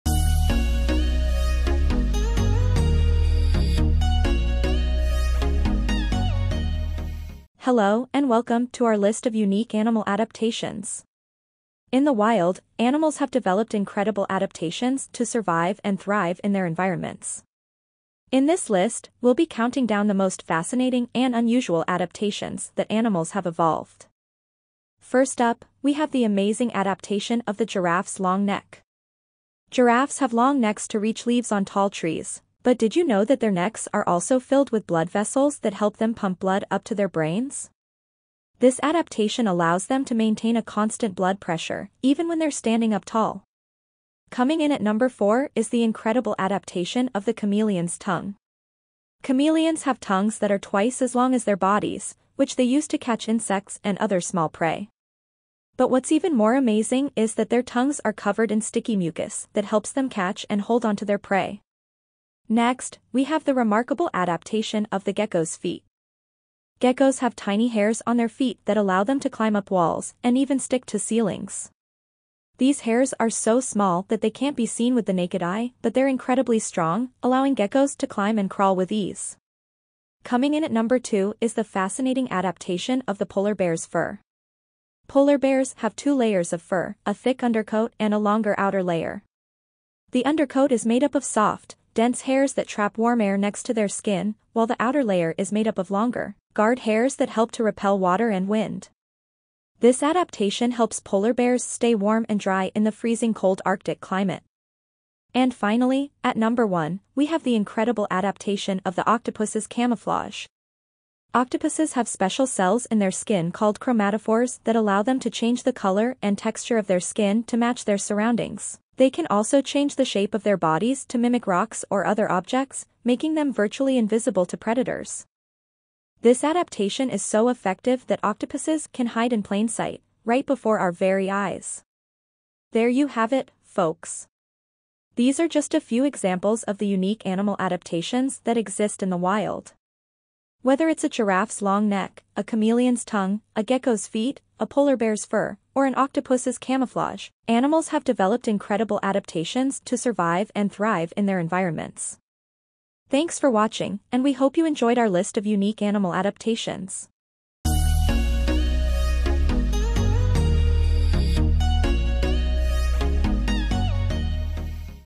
Unique Animal Adaptations
We'll also learn about the science behind these adaptations and how they help

In this video, we take a look at the most unique animal adaptations on the planet. From the chameleon's ability to change color to the armadillo's armored shell, these animals have evolved some amazing ways to survive in their environment.